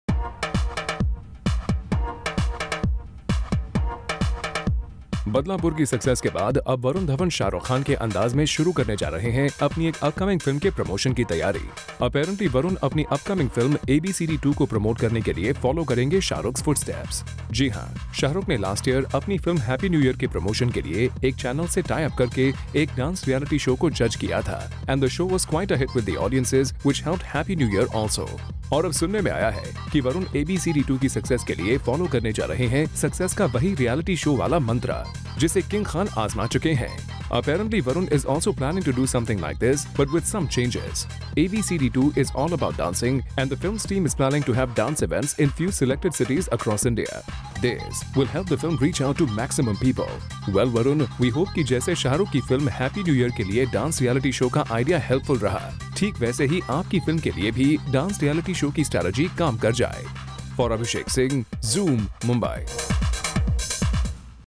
Varun Dhawan Follows Shahrukh Khan's Footsteps | EXCLUSIVE
Varun Dhawan follows Shahrukh Khan's footsteps to promote movie ABCD 2